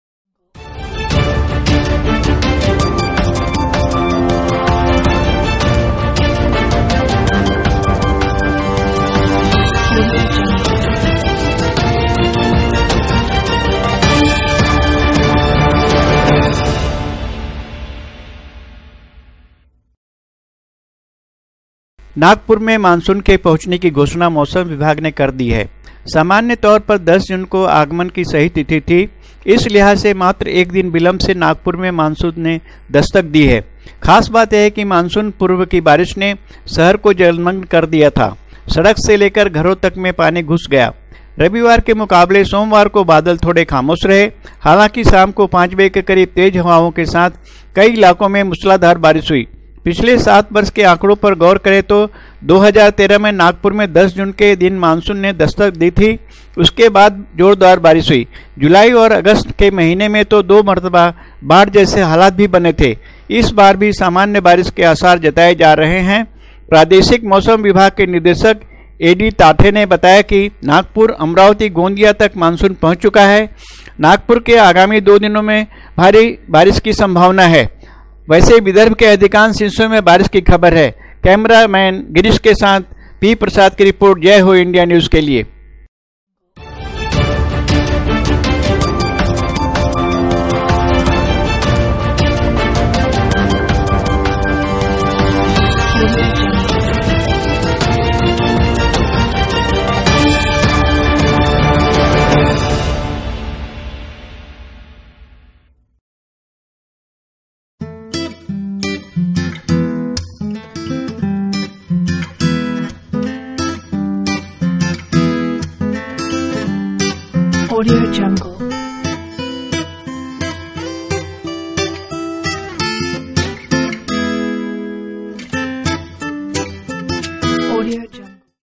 जय हो इंडिया न्यूज़ , नागपुर - नागपुर मानसून आगमन , बारिश जोर आने सभावना
वेब्सीडे
यूटूब